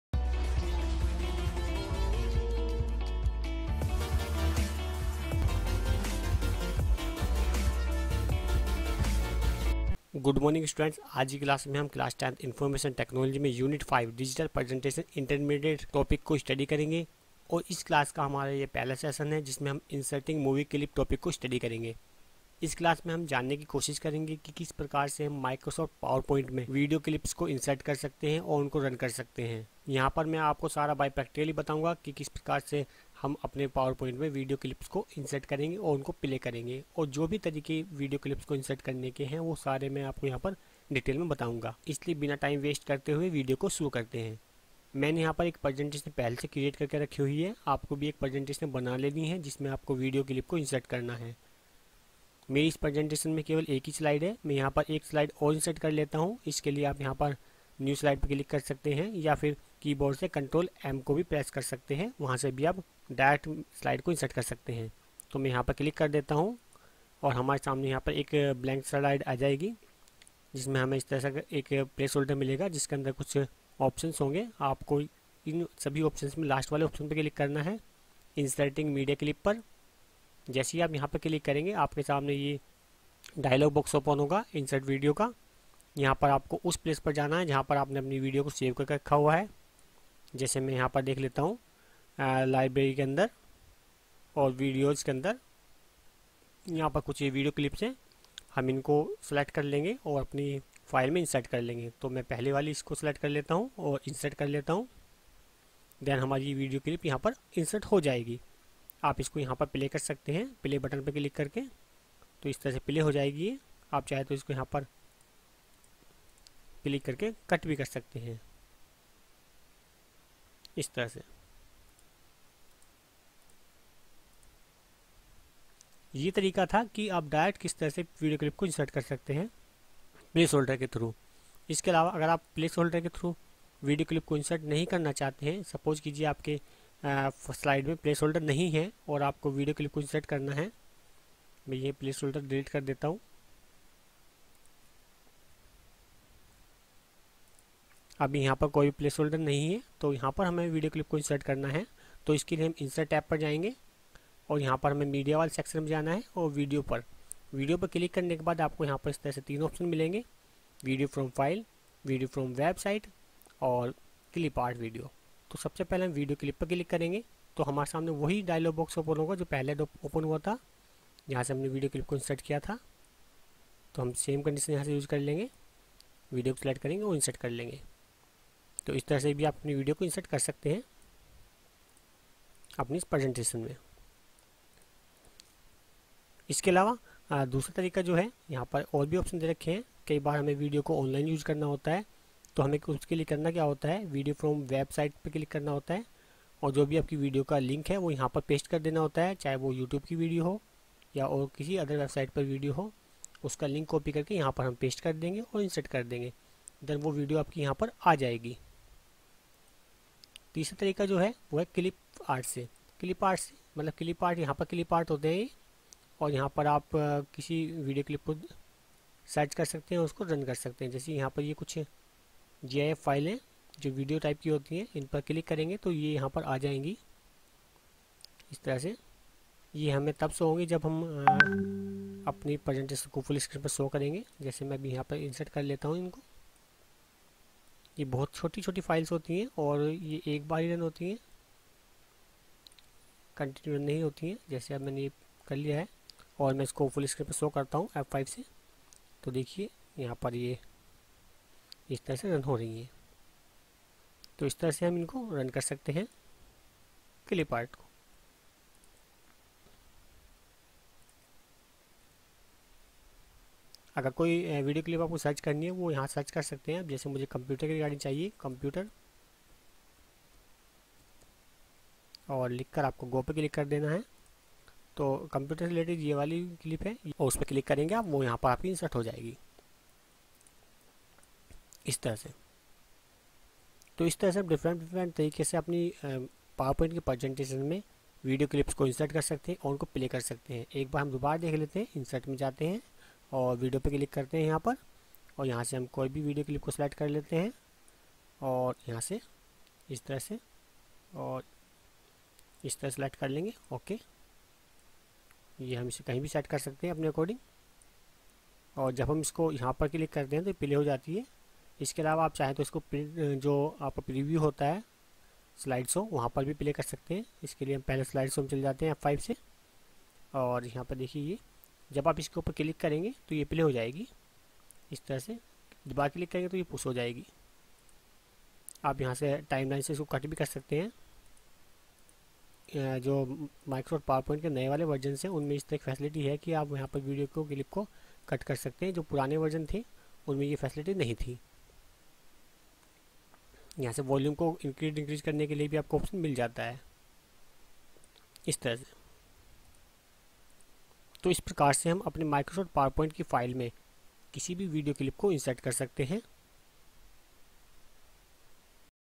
SESSION-1: Inserting Movie Clips | Unit- Digital Presentation (Intermediate), Information Technology
In this session class 10 CBSE board students will learn Information Technology NSQF Level-2, Unit- Digital Presentation (Intermediate), Session-1 Inserting Movie Clips.
This based on Microsoft PowerPoint, in this class students, know how to insert video clips in Microsoft PowerPoint presentation. and what are the way to using video clips in PowerPoint or presentation?

Class-10 CBSE Board
Subject: Information Technology NSQF Level-2
Subject Code: 402
Unit-5: Digital Presentation
Session-1: Inserting Movies Clips

Fill in the blanks:
1. Presentation allows the user to add _____ _______ to the presentation in order to make the presentation colourful and meaningful.
2. You can use two options to insert a movie clip the _________________ &
3. The two options available under Movie option are _____________________ &
4. Movie option is available in ______________ group under the Insert tab.

Important Questions:
Q.1: Write the steps to insert Video clips in PowerPoint Presentation.

Answer:
1. Movie Clips,
2. Using Insert Tab, Using Title and Content Layout Option.,
3. Movie From File, Movie from Clip Organizer.
4. Media